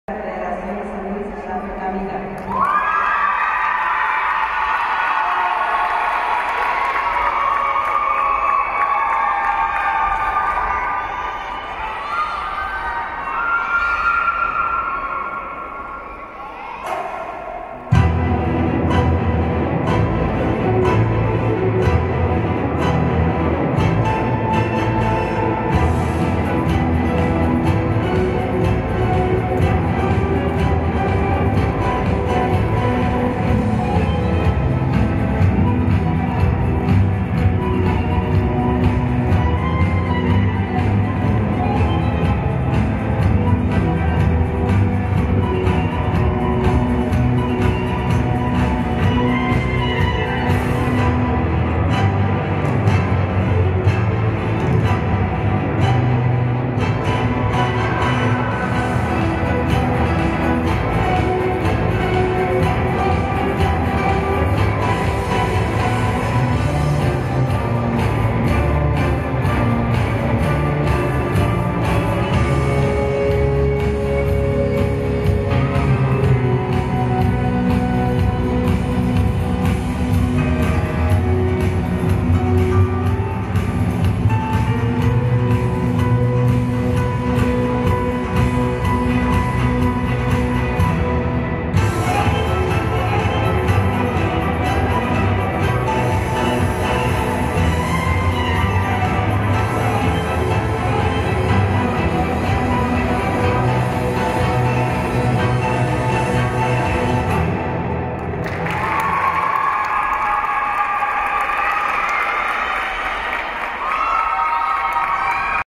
CAMILA SCHAFFER CUERDA SANLUISEÑA
TORNEO FEDERATIVO A & B 2019 - todas categorias
Sede : Polideportivo Ave Fènix - Juana Kolsay - San Luis
Octubre 31 - Noviembre 1ª y 2